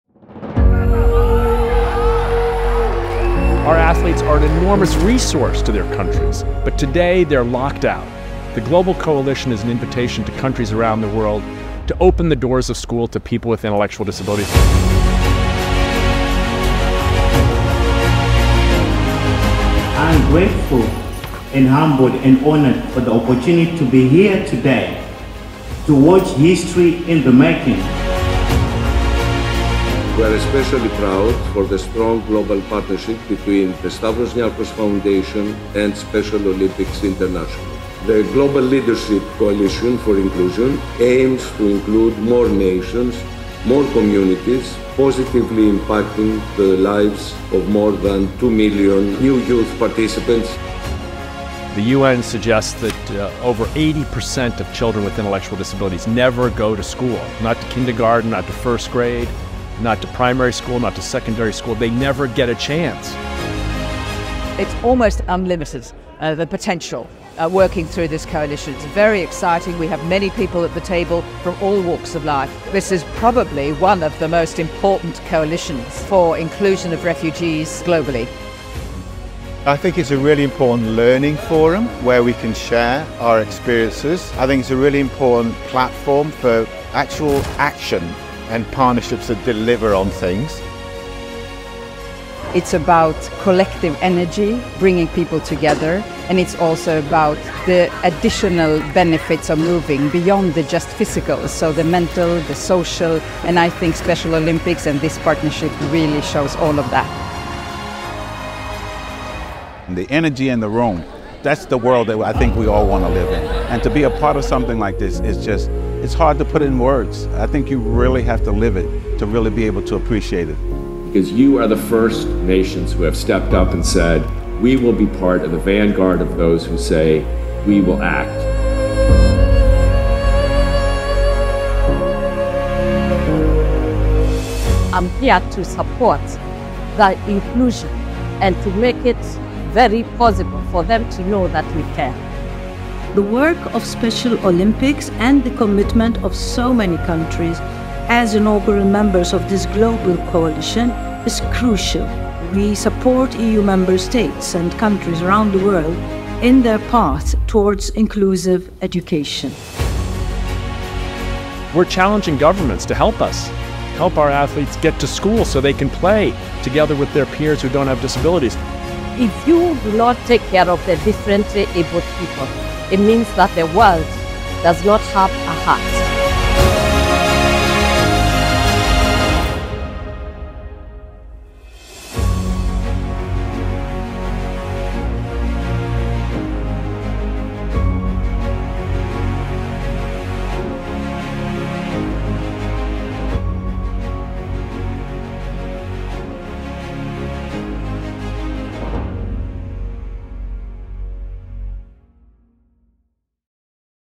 Special Olympics Global Leadership Coalition for Inclusion
As world leaders convene at the UNGeneralAssembly, @specialolympics is thrilled to showcase the Special Olympics Global Leadership Coalition for Inclusion, expanding inclusive education & sports opportunities to 2m+ people.